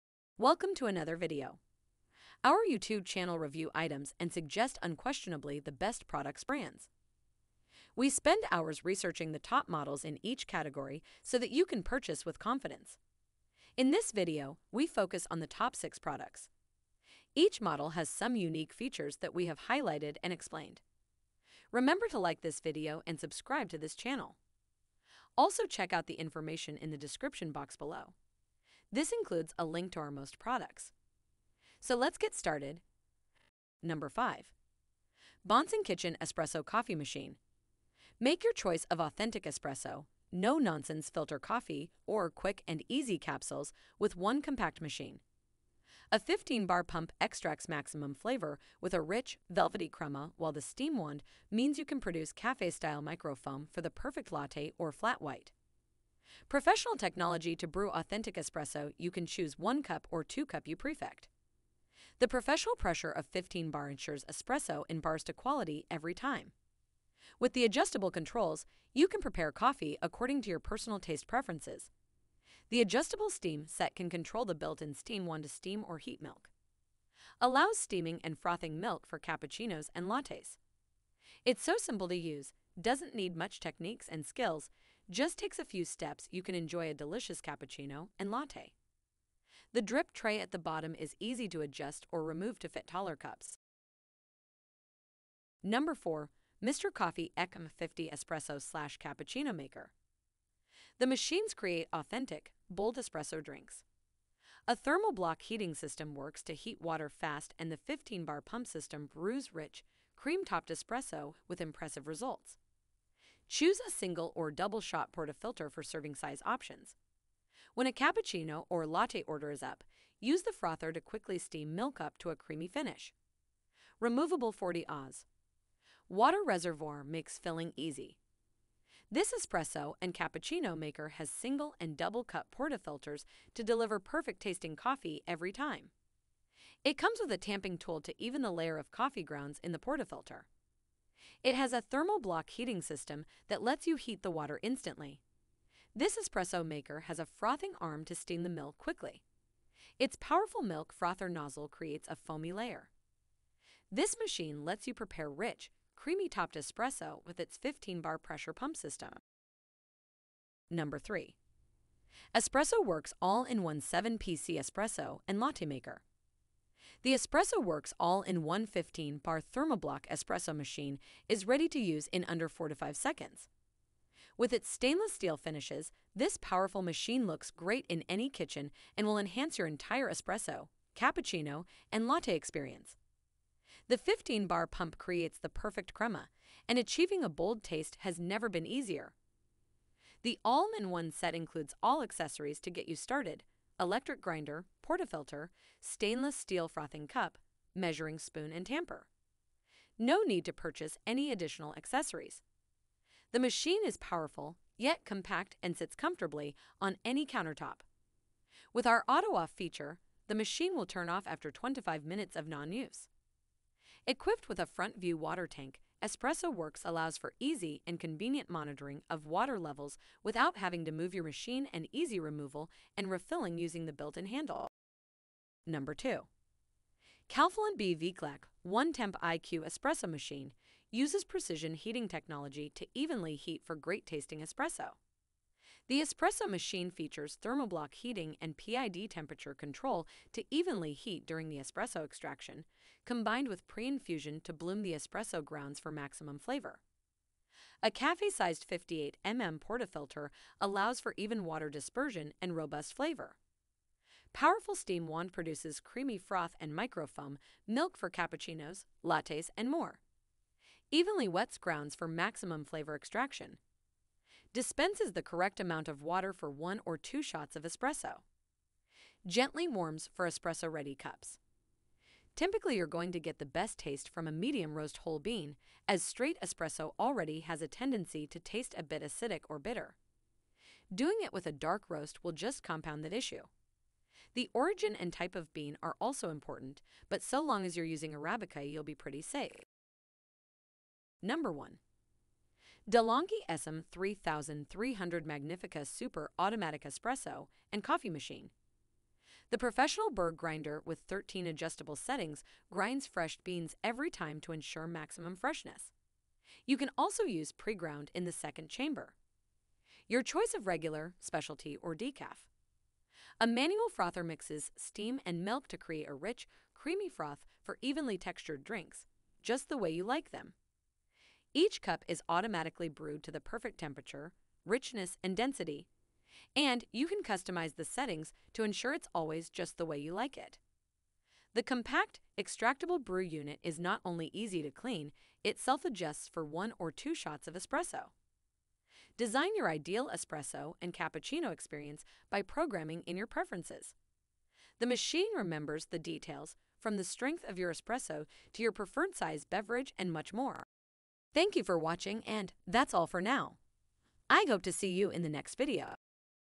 The top 5 espresso machines you can buy today
We've played with just about every espresso machine out there. Just like you, we've ridden the waves of hype and excitement that come with new tech and modern designs. At the end of the day, we still recommend the top 5 as the best end-game espresso machine.

► Links◄
➜ 5. Bonsenkitchen Espresso Coffee Machine
➜ 4. Mr. Coffee ECMP50 EspressoCappuccino Maker
➜ 3 EspressoWorks All-In-One 7 Pc Espresso & Latte Maker
➜ 2. Calphalon BVCLECMP1 Temp iQ Espresso Machine
➜ 1. De'Longhi ESAM3300